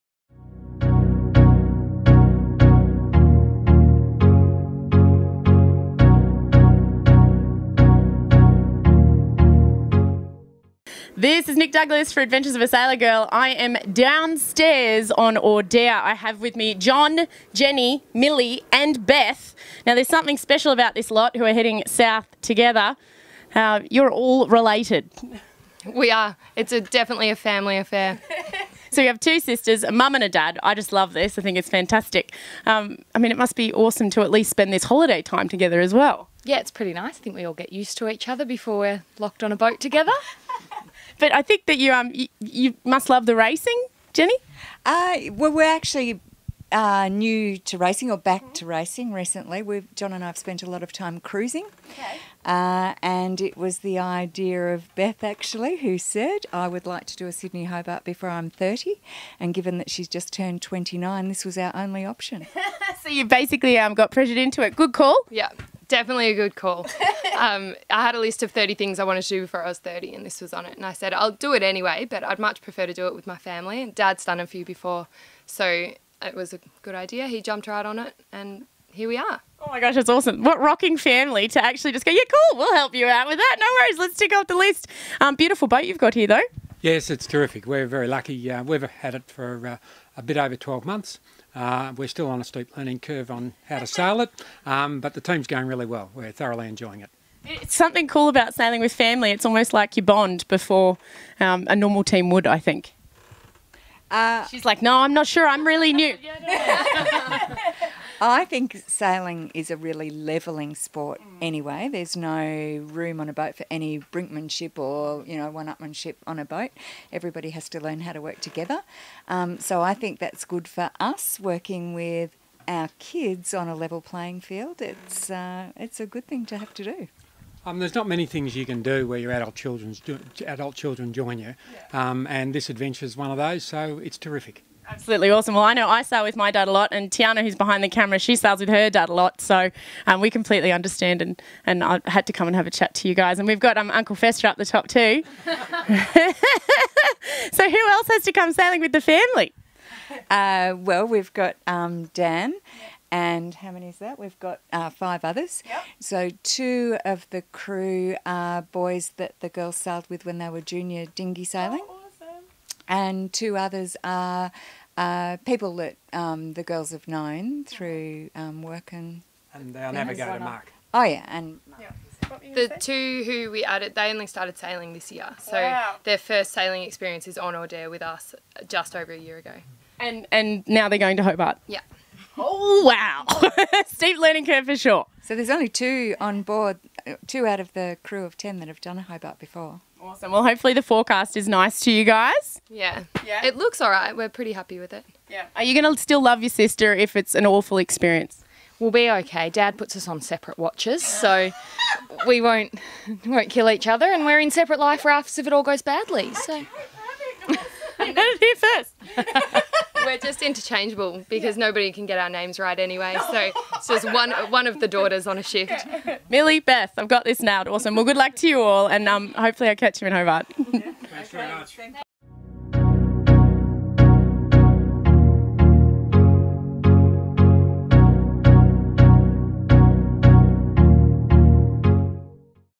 Rolex Sydney Hobart 2018: Cain Family - Audere
My chat with Beth, Milly, Jenny and John Cain down below on their yacht Audere. Amazing to talk to some experienced sailors, and some not so experienced, from the same family, who were all keen to head to Hobart together... all the name of ticking off items on a list! Very cool...